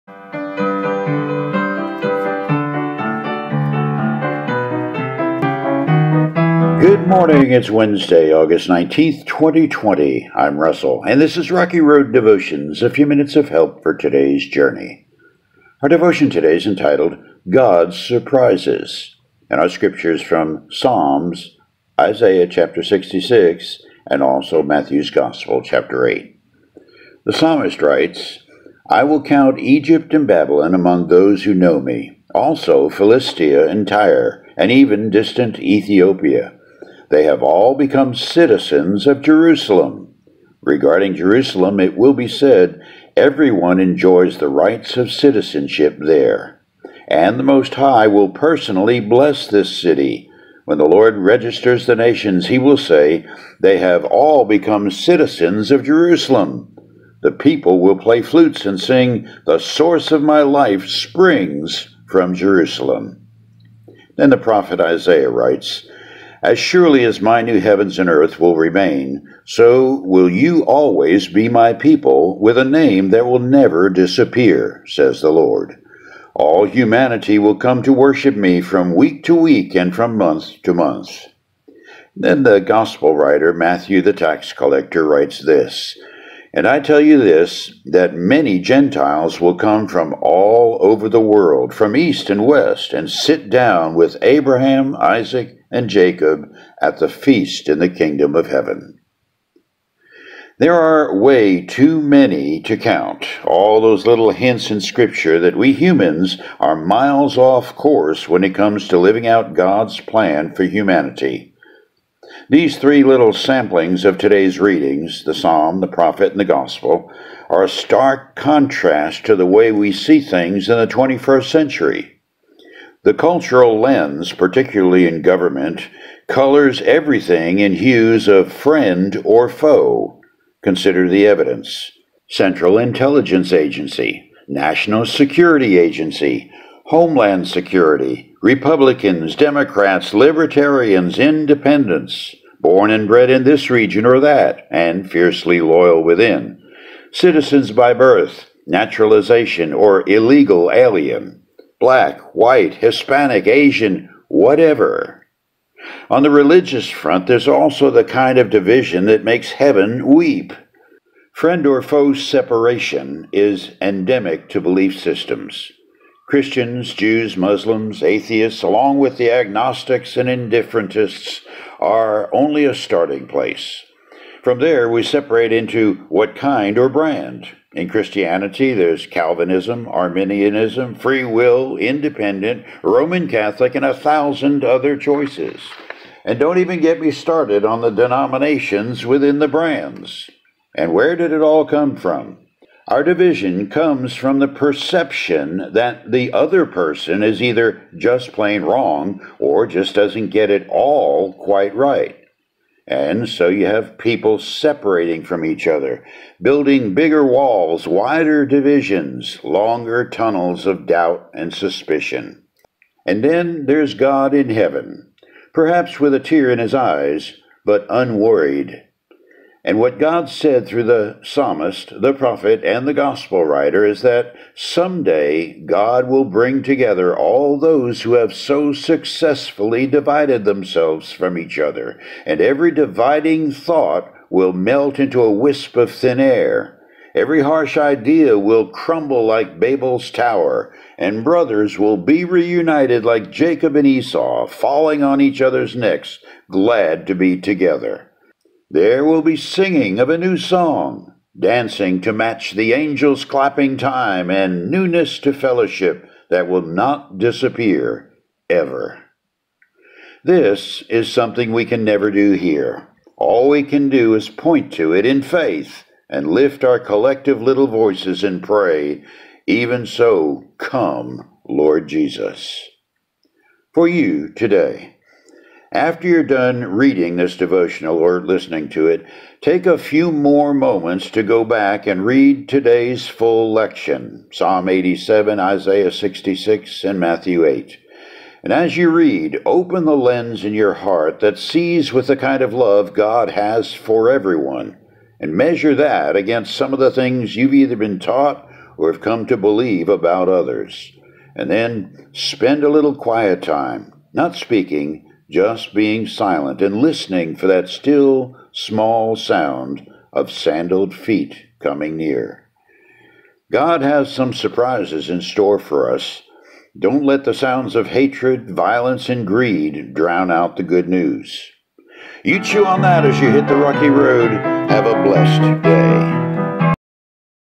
8 19 20 God's Surprises
The surprise (and blessing) of finding out you were wrong.